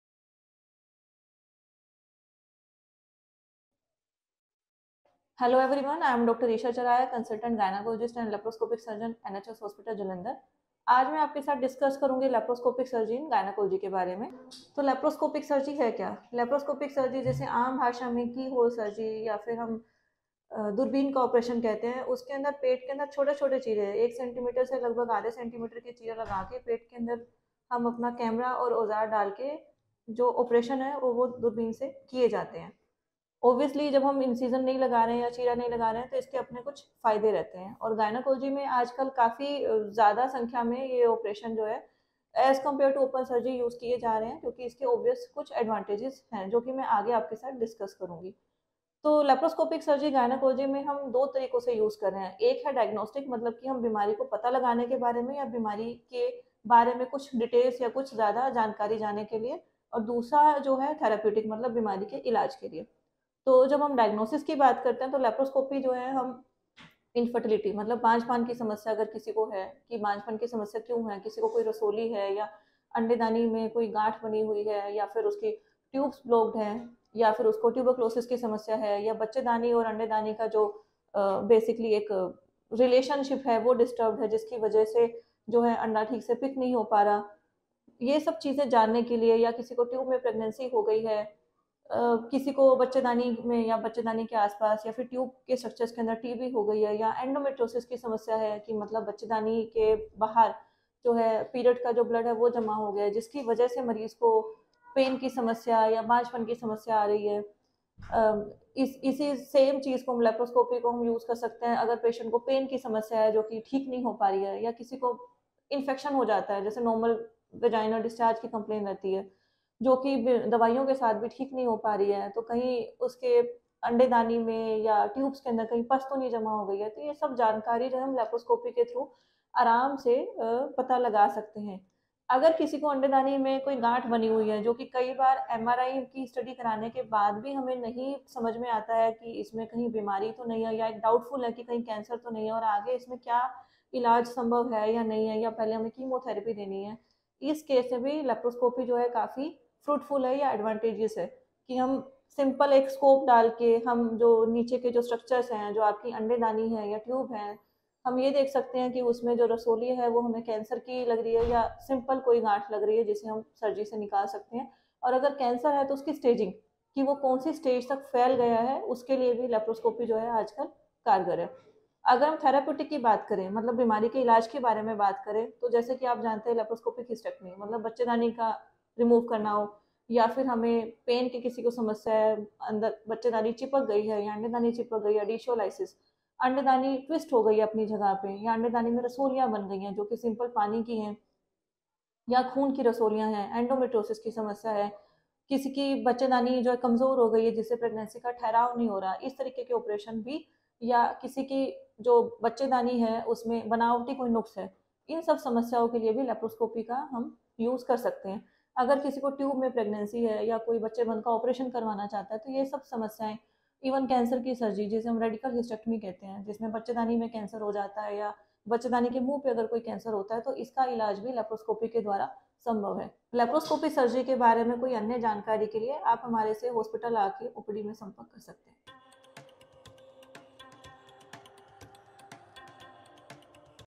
Laparoscopic Surgery for Female Health | Endometriosis, Infertility & More
Welcome to our informative video featuring a renowned gynaecologist as she delves into the world of laparoscopic surgery and its role in addressing various female health issues. In this comprehensive discussion, you'll gain valuable insights into how laparoscopic surgery can tackle infertility, endometriosis, and more.

Topics Covered:
- The benefits of laparoscopic surgery for female problems.
- How laparoscopy can help with infertility issues.
- Treating endometriosis through minimally invasive techniques.
- The process of egg retrieval for fertility treatments.